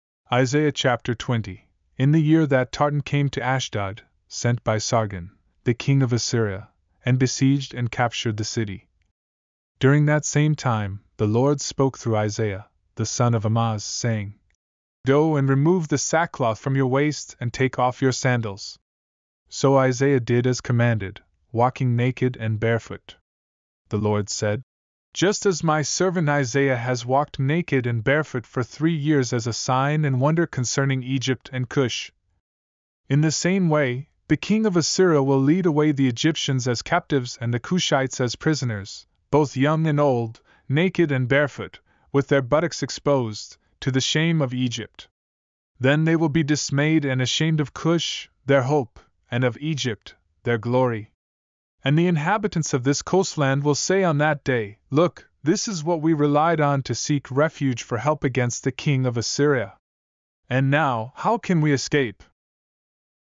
Isaiah Chapter 20 - The Holy Bible (KJV) "audiobook - read along" ✝️
Isaiah Chapter 20 of 66 The Holy Bible (KJV)

Isaiah Chapter 20:  In the year that Tartan came to Ashdod, sent by Sargon, the king of Assyria, and besieged and captured the city, During that same time, the LORD spoke through Isaiah, the son of Amoz, saying, "Go and remove the sackcloth from your waist and take off your sandals." So Isaiah did as commanded, walking naked and barefoot. The LORD said, "Just as my servant Isaiah has walked naked and barefoot for three years as a sign and wonder concerning Egypt and Cush, In the same way, the king of Assyria will lead away the Egyptians as captives and the Cushites as prisoners, both young and old, naked and barefoot, with their buttocks exposed, to the shame of Egypt. Then they will be dismayed and ashamed of Cush, their hope, and of Egypt, their glory. And the inhabitants of this coastland will say on that day, 'Look, this is what we relied on to seek refuge for help against the king of Assyria! And now, how can we escape?'"